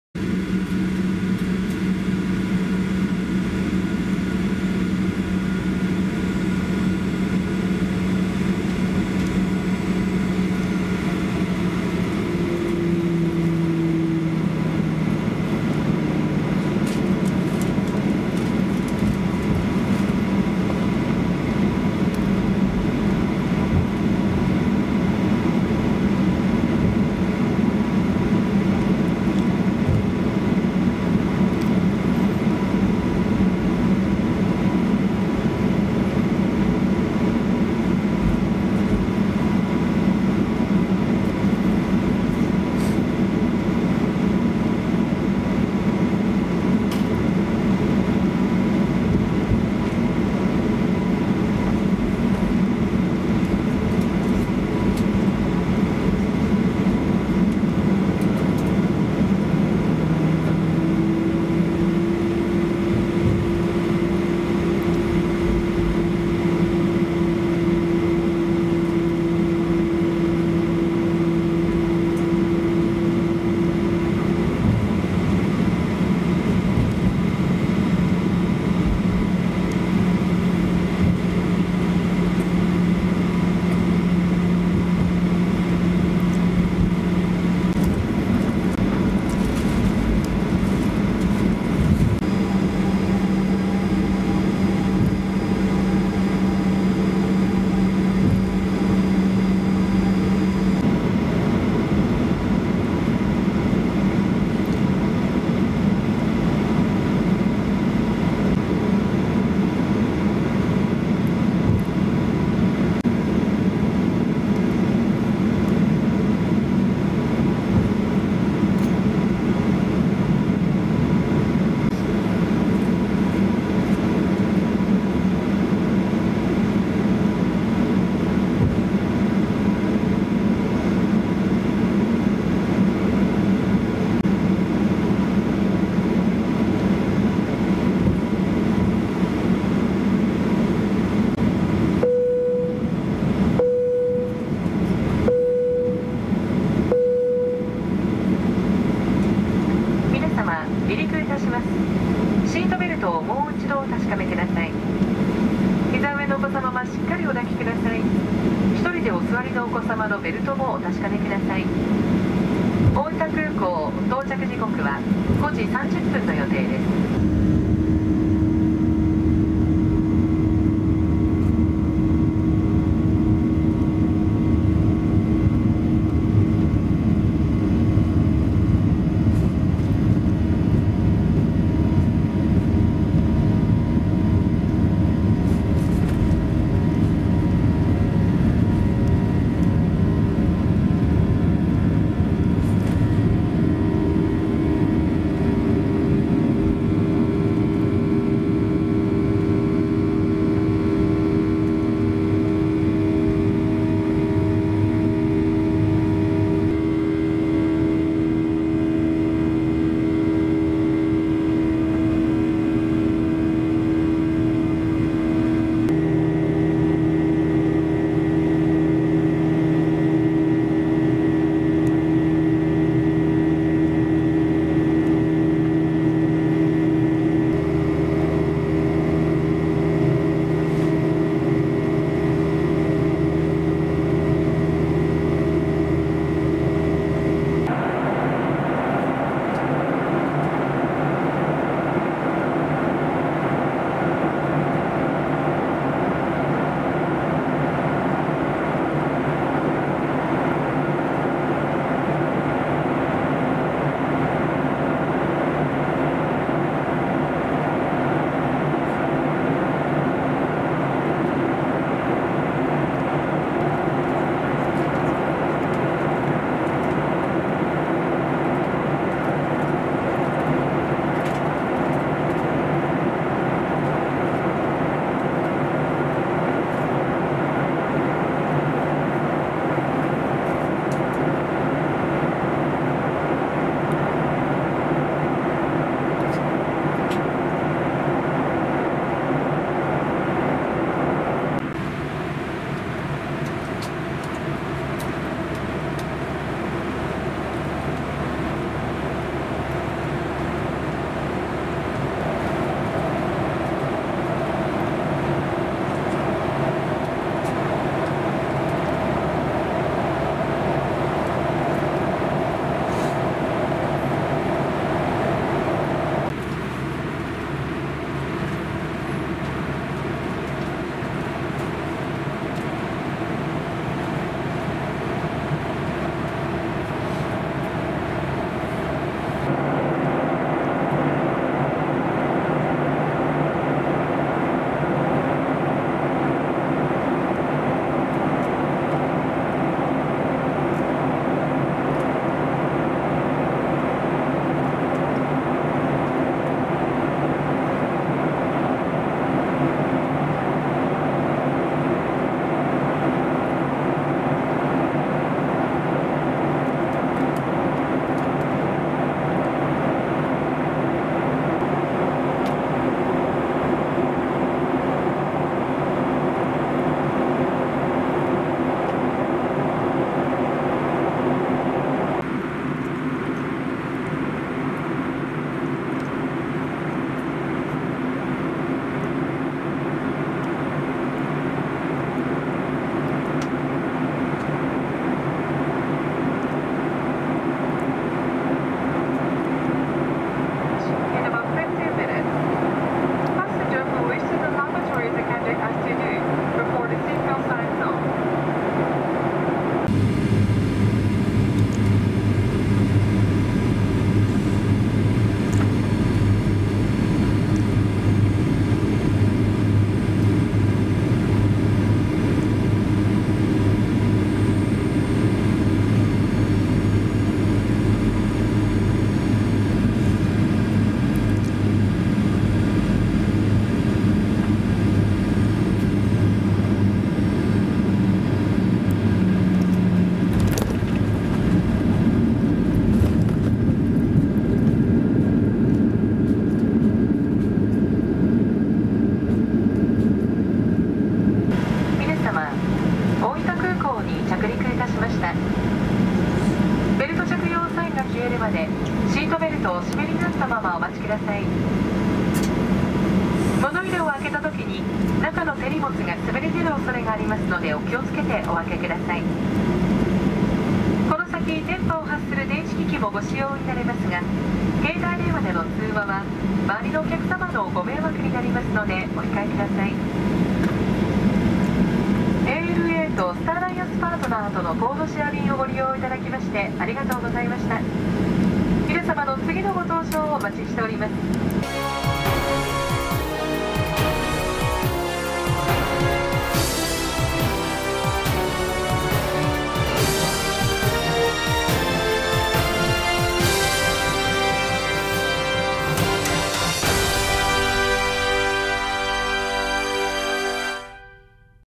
羽田空港→大分空港ダイジェスト／SFC取得を目指す旅／ANAプレミアムクラス
2019年3月17日　ANA東京国際空港（羽田）→大分空港SFC取得を目指す旅～ANAプレミアムクラスより撮影したタキシング、離陸、食事、着陸の映像です。

Everyday Update!

JR全線完全乗車
2001年 JGC(JALグローバルクラブ)
2019年 SFC(ANAスーパーフライヤーズ)修行中